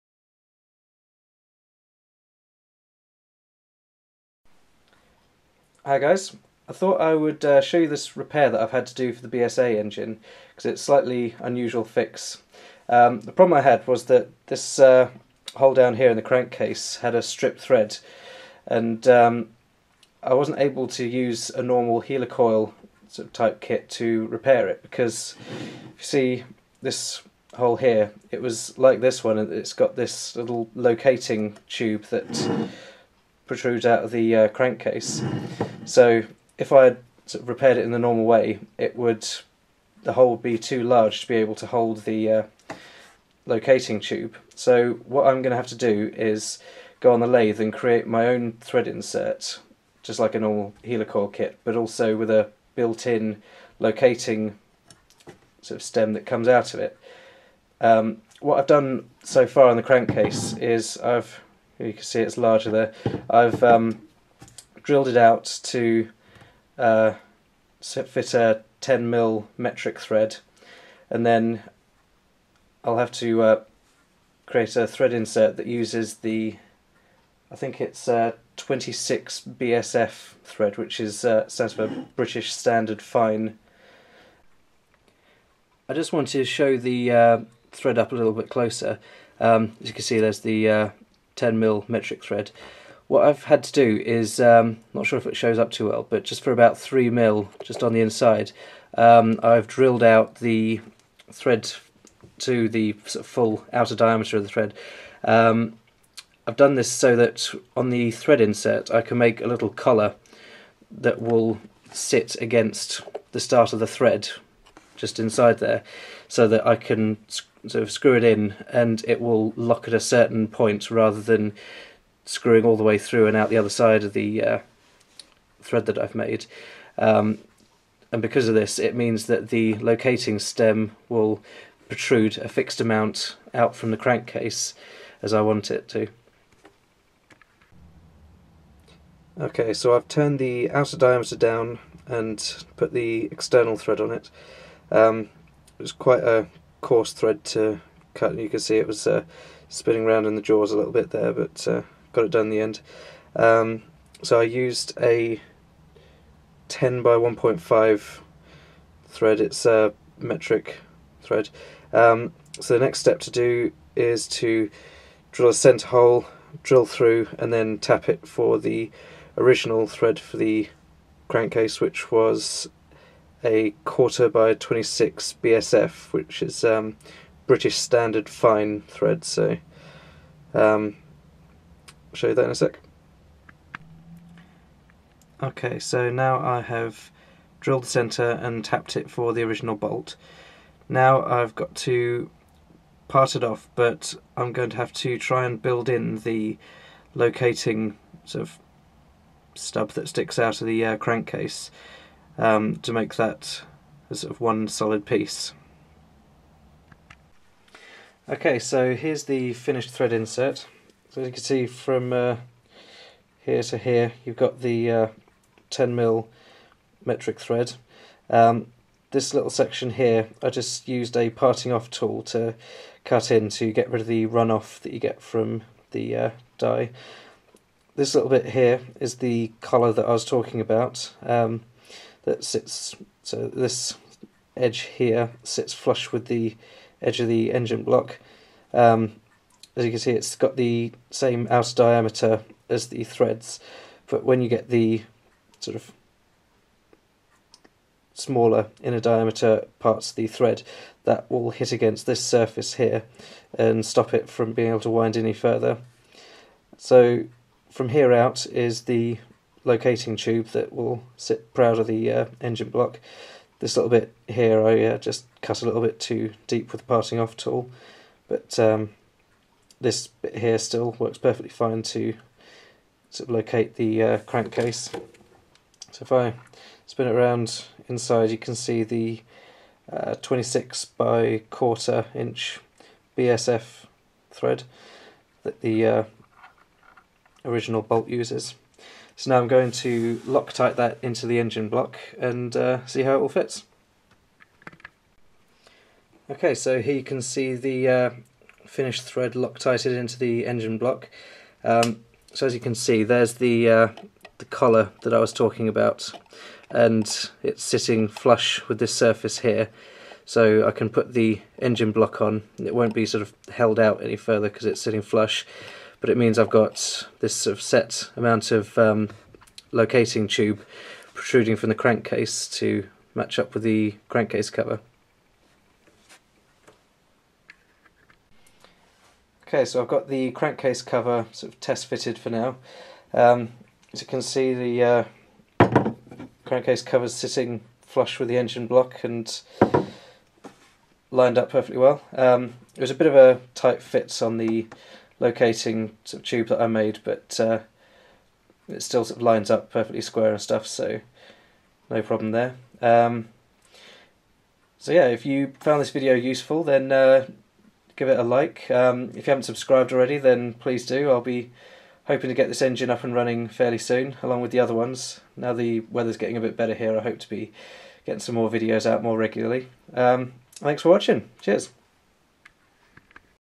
Homemade Thread Repair - BSA Engine
A video of how I made my own thread insert repair for my BSA engine, including the crankcase cover locating tube.